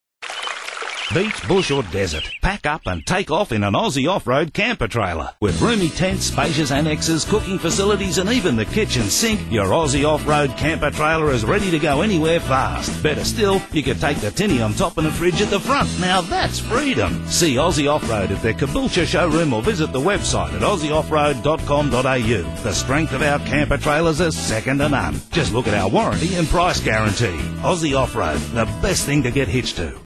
Camper Trailers Brisbane & Sunshine Coast
Check out one of our TV ads for camper trailers that ran in Brisbane and the Sunshine Coast in 2007 - an oldie but a goodie!